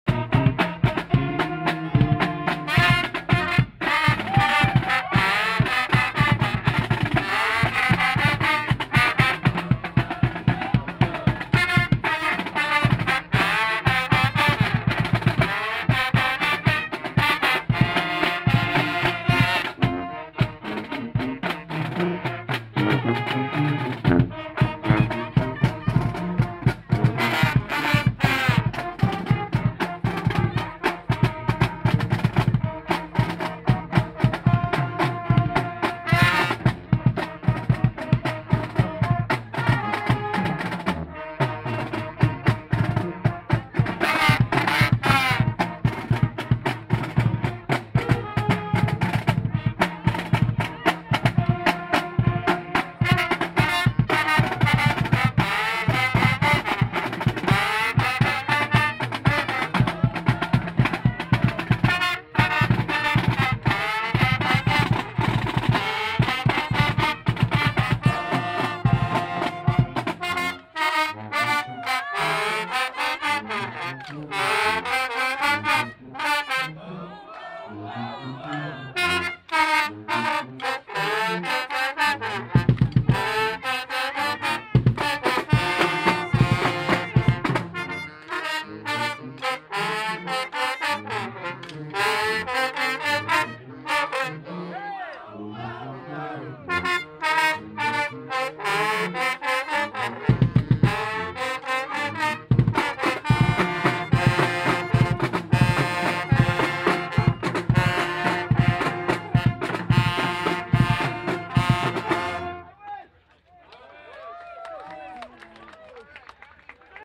Stanford Marching Band
Stanford marching band performing at GOALIE (Graduate Orientation and Lunch Information Event) on September 18, 2015